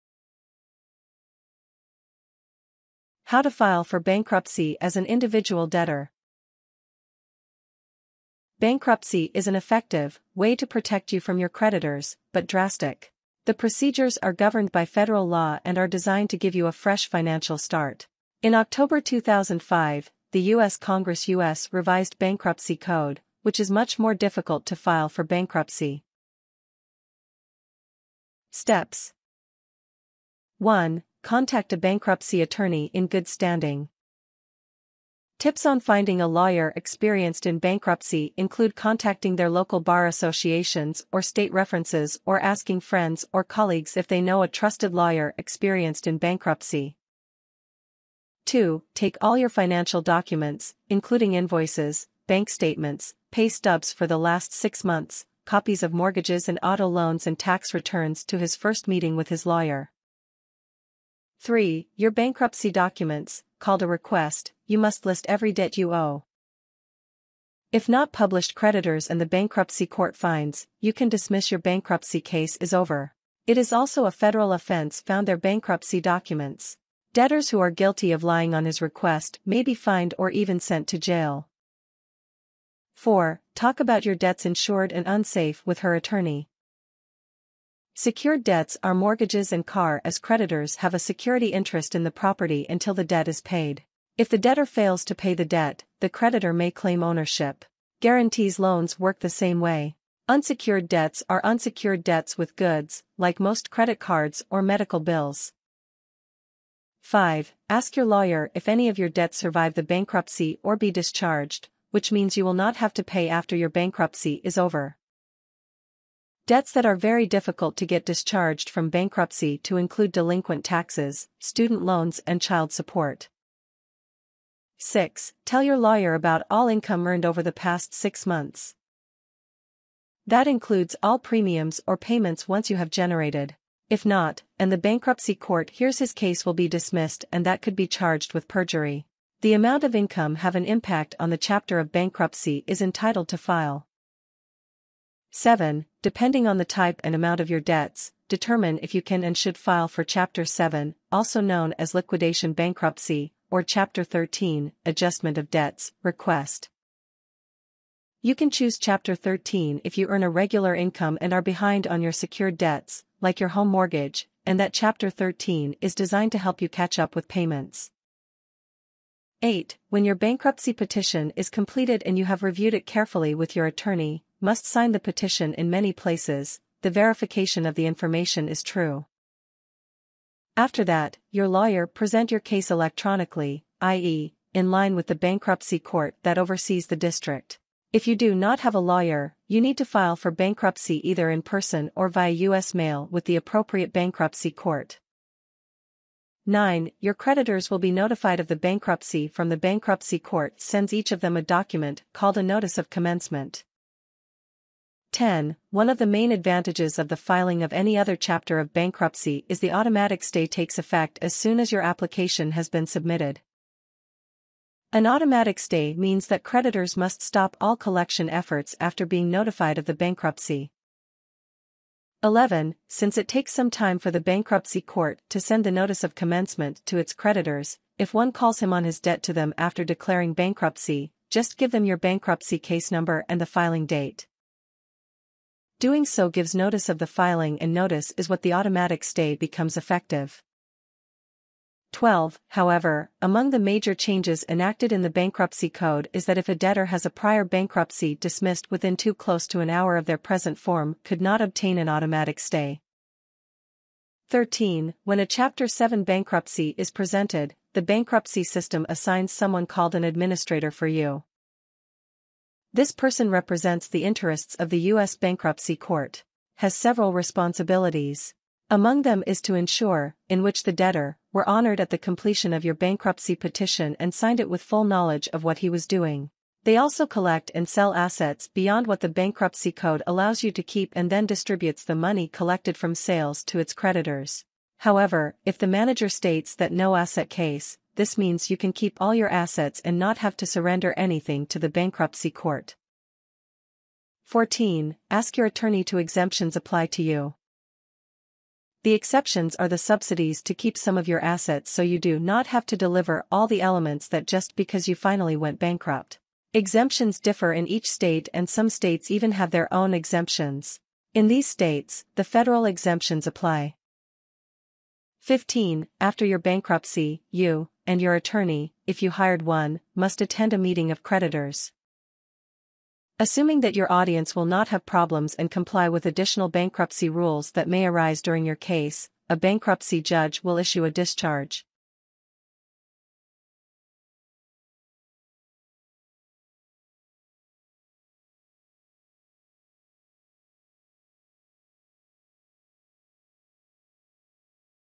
Guide to Filing for Bankruptcy as an Individual Debtor: Steps, Tips, and Important Considerations
In this video, you will learn how to file for bankruptcy as an individual debtor without losing your house. Filing for bankruptcy can provide protection from creditors and give you a fresh financial start. It is important to find a bankruptcy attorney who is experienced in this area of law. Gather all your financial documents, such as invoices, bank statements, pay stubs, and tax returns, for your first meeting with your lawyer.    When filling out your bankruptcy documents, called a request, make sure to list every debt you owe. Failure to include any creditors may result in your case being dismissed. It is also a federal offense to provide false information on your bankruptcy documents.    Discuss with your lawyer the distinction between secured and unsecured debts. Secured debts, such as mortgages and car loans, have a security interest in the property until the debt is paid. Unsecured debts, like credit card or medical bills, do not have any collateral attached to them.    Find out from your lawyer which debts can be discharged in bankruptcy and which ones may still need to be paid. Certain debts, such as delinquent taxes, student loans, and child support, may be difficult to discharge.    Provide your lawyer with accurate information about your income over the past six months. This will determine the type of bankruptcy you are eligible to file for.    Depending on the type and amount of your debts, your lawyer will help you decide whether to file for Chapter 7 or Chapter 13 bankruptcy. Chapter 7 is also known as liquidation bankruptcy, while Chapter 13 is an adjustment of debts.    Once your bankruptcy petition is completed and reviewed with your attorney, you will need to sign it in multiple places to verify the information is true. Your lawyer will then file your case electronically with the appropriate bankruptcy court.    After filing for bankruptcy, your creditors will be notified by the court through a document called a Notice of Commencement. This notification triggers an automatic stay, which means creditors must stop all collection efforts.    If a creditor contacts you after you have declared bankruptcy, simply provide them with your bankruptcy case number and filing date to inform them of the automatic stay.    It is important to note that if you have had a prior bankruptcy dismissed recently, you may not be eligible for an automatic stay.    In a Chapter 7 bankruptcy, an administrator will be assigned to your case by the bankruptcy court. Their responsibilities include ensuring that you completed your bankruptcy petition and understanding its implications. They may also collect and sell assets that exceed the allowable exemptions and distribute the proceeds to your creditors.    Discuss the exemptions that may apply to you with your attorney. Exemptions allow you to keep certain assets so you do not have to surrender everything in the bankruptcy process. Exemptions vary by state, and some states have their own exemptions, while others follow federal exemptions.    After your bankruptcy is completed, it is important to continue working with your attorney to ensure compliance with any remaining obligations. Remember to follow their guidance and make any necessary payments.    Knowing when to file for bankruptcy is a personal decision, but seeking legal advice and understanding the process can help you make an informed choice.